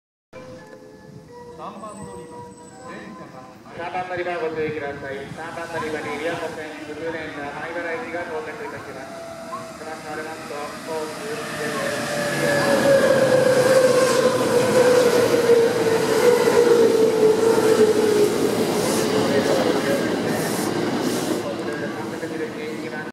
JR西日本 221系 B4編成 山科駅
2019年撮影
普通　米原行き
東海道本線　山科駅ホーム京都方から